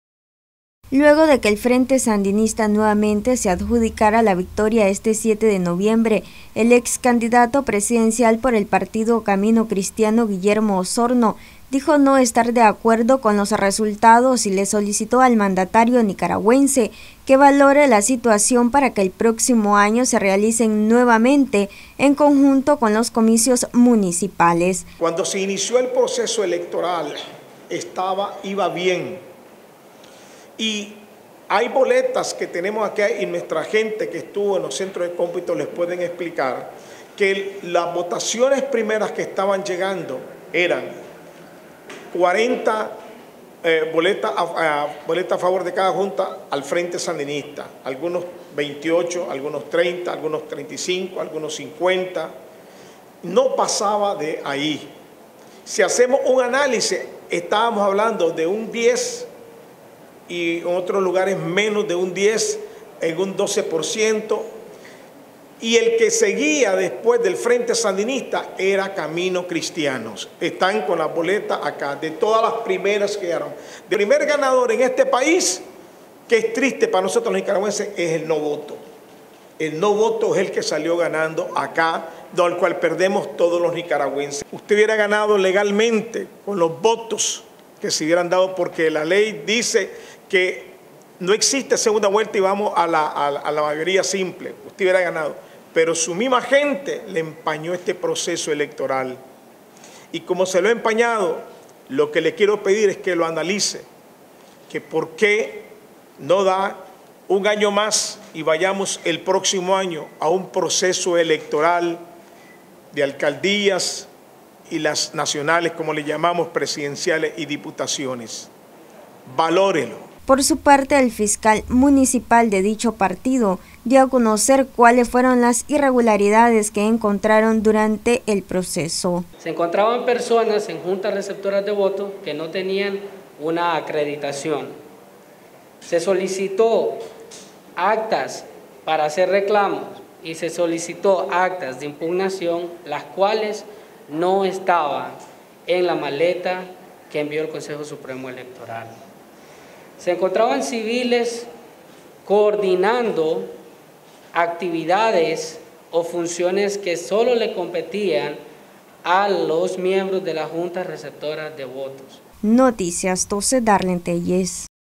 Partido Camino Cristiano reclama el segundo lugar en las elecciones de Nicaragua
Noticias12Nicaragua Partido Camino Cristiano le solicita al gobierno realizar el próximo año las elecciones presidenciales en conjunto con las municipales, ya que no están de acuerdo con los resultados actuales.